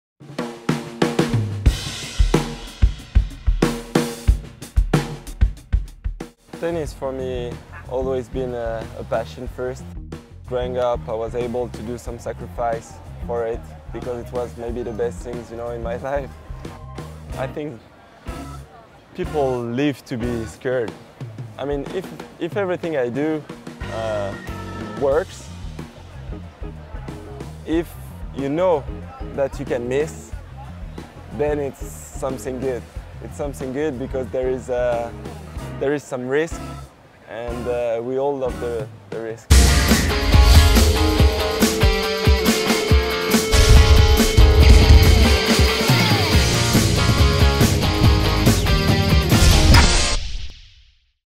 Jo-Wilfried Tsonga talks pressure and risk
Jo opens up about tennis, inspiration, and risk.